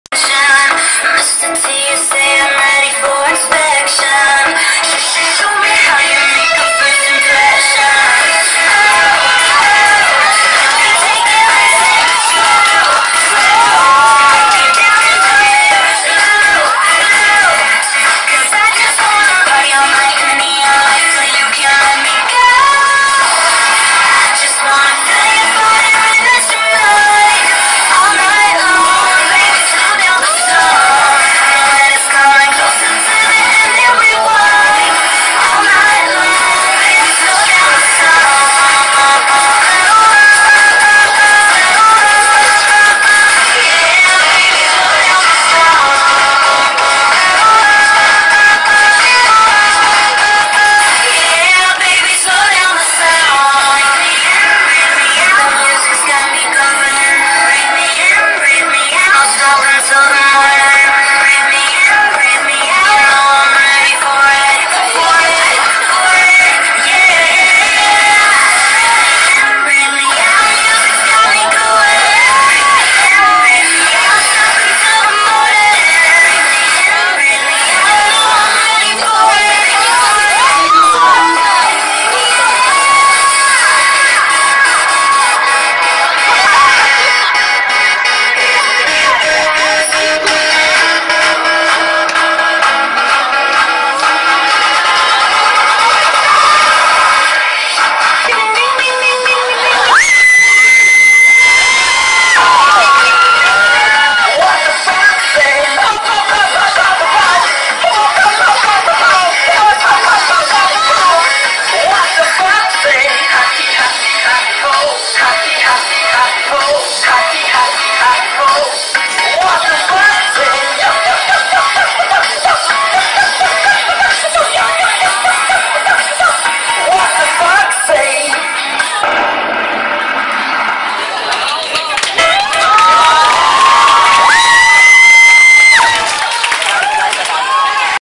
CHRISTMAS PARTY (Troop 347)
a random performance that was prepared 3 hours before the show started.